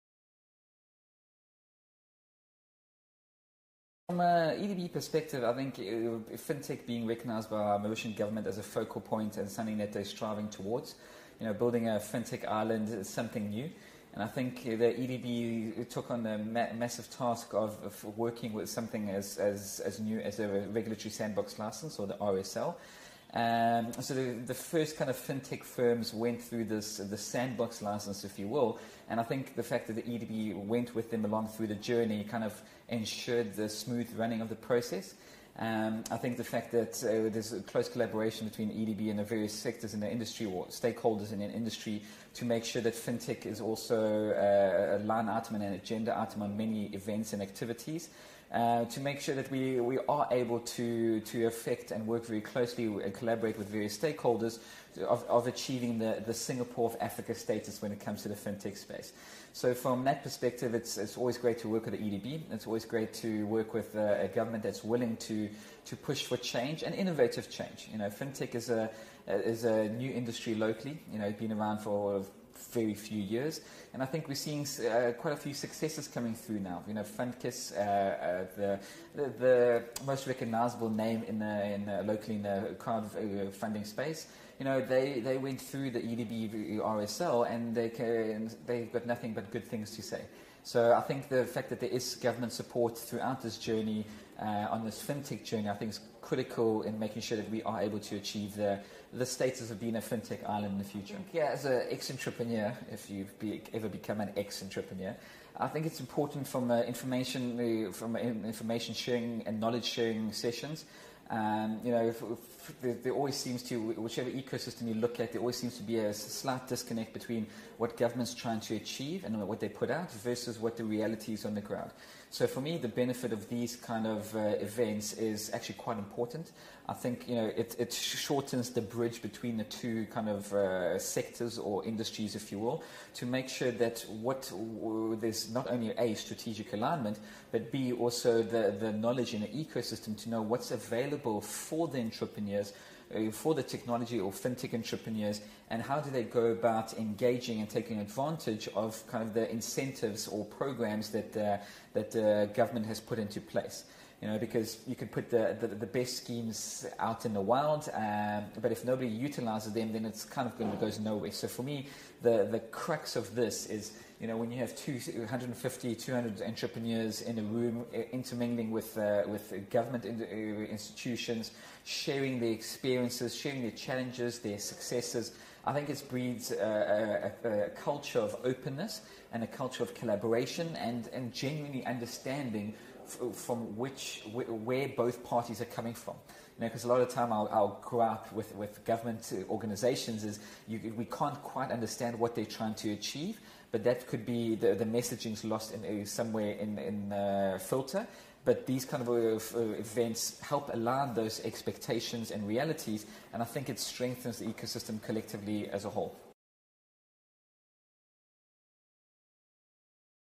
Michal Szymanski, CEO, Mauritius Africa FinTech Hub (MAFH)
Watch the interview of Michal Szymanski, CEO, Mauritius Africa FinTech Hub (MAFH), in the context of the Workshop "Building Resilience through SMEs and Entrepreneurship".